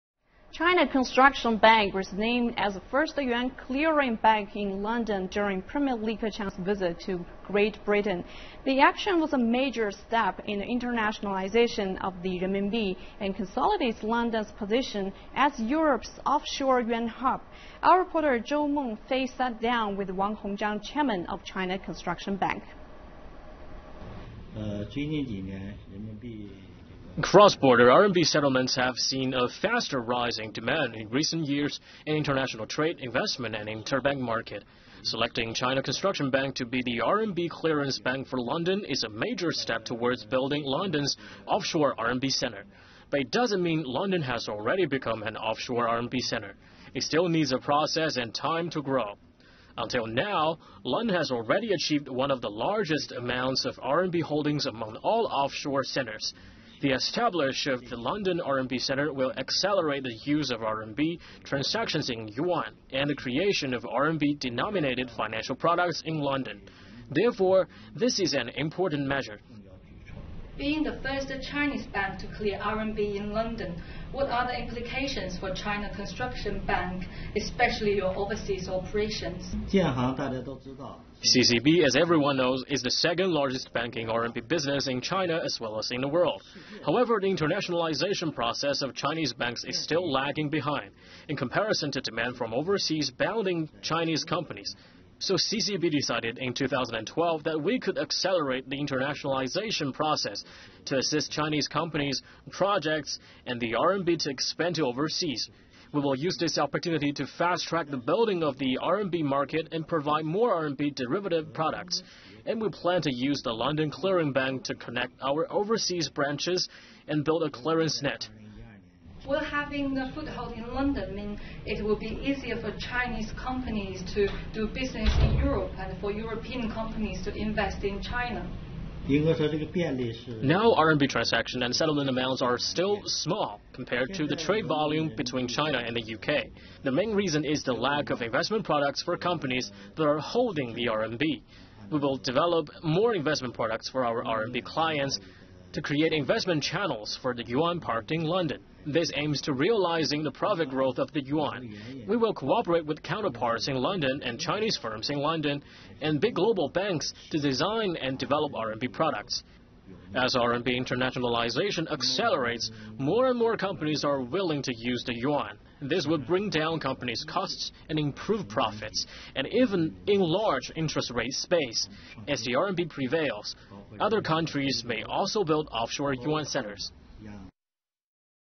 CCB appointed as yuan-clearing bank in London
China Construction Bank was named as the first yuan-clearing bank in London during Premier Li Keqiang's visit to Great Britain. The action was a major step for the internationalization of the renminbi, and consolidates London's position as Europe's offshore yuan hub.